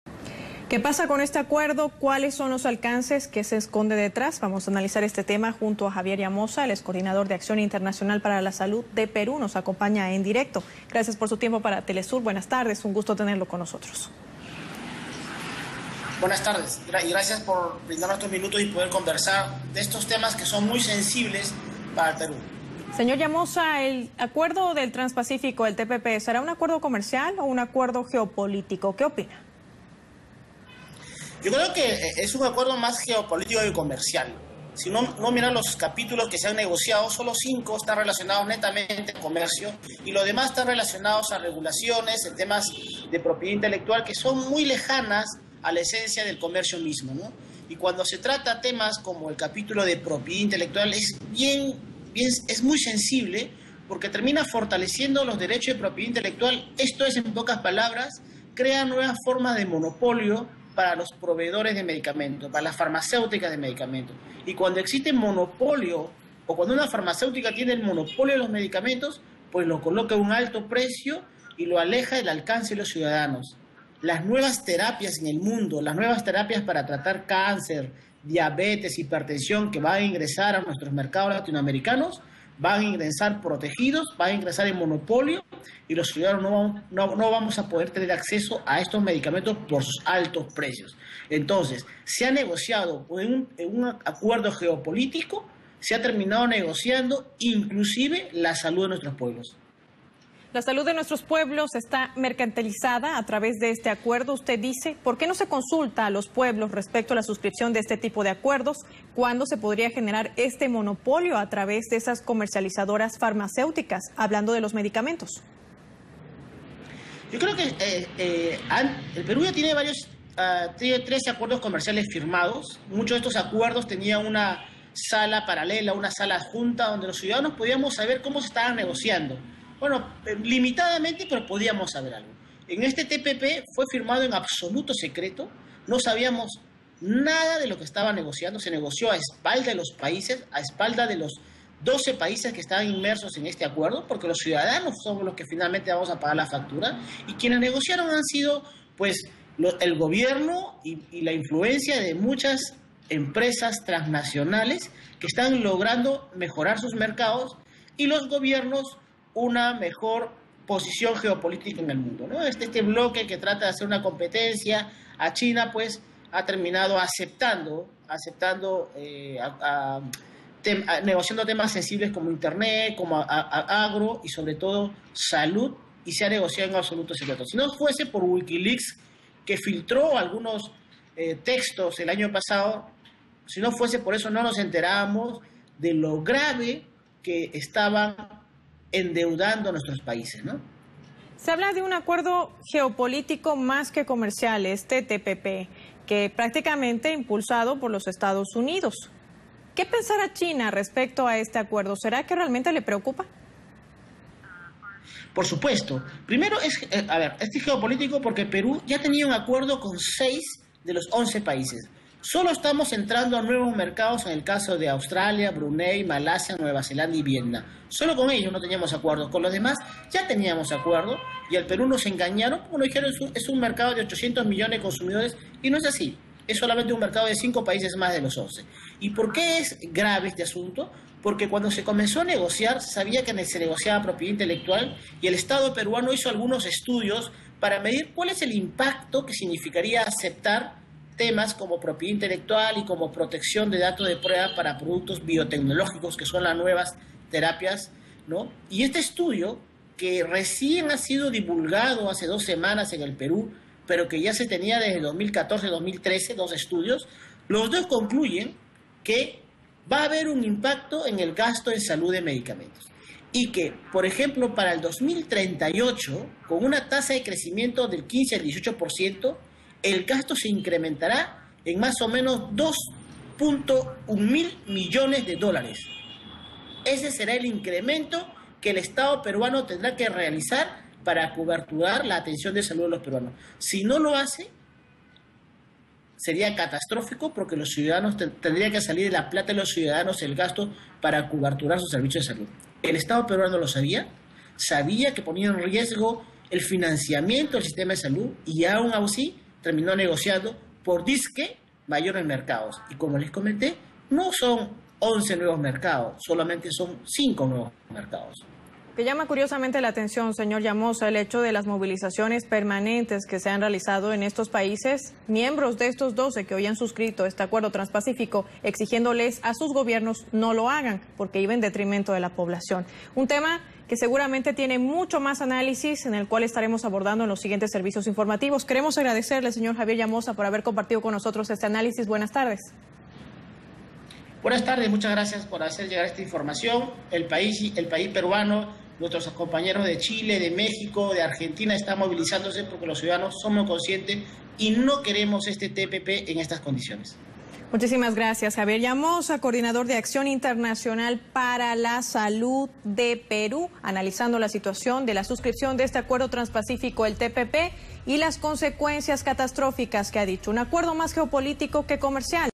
Llamoza: El TPP es más un acuerdo geopolítico que comercial
Entrevista a Javier Llamoza de AIS-RedGE en la cadena internacional de Telesur sobre los impactos del TPP